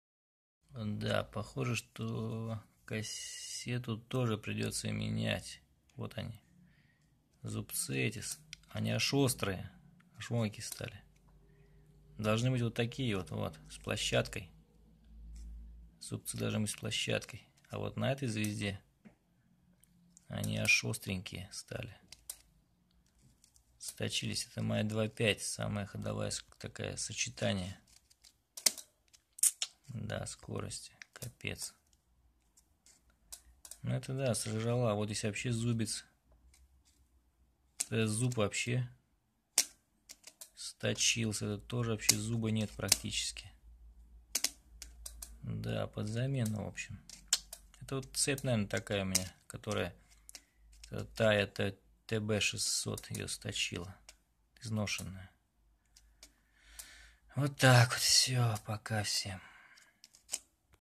Износ зубьев на кассете
Пятая звёздочка (четвёртая сверху) износилась вхлам